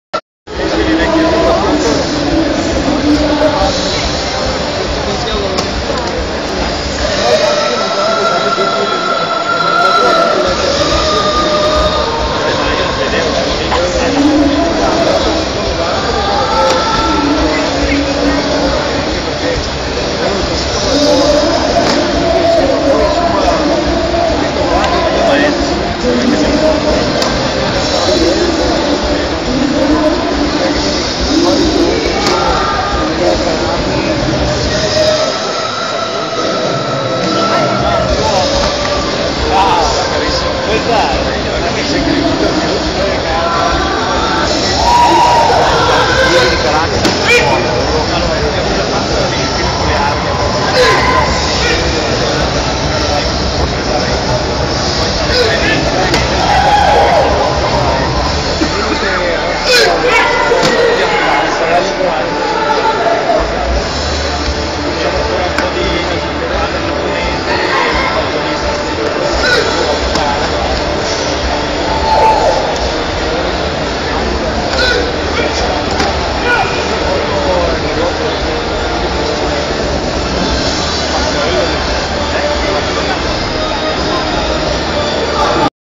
Randori San no Kata
Randori San no Kata eseguito dal Maestro Sergio Mor Stabilini (8° Dan).
Festival dell'Oriente - Parco Esposizioni Novegro
Segrate (MI)
25 Aprile 2013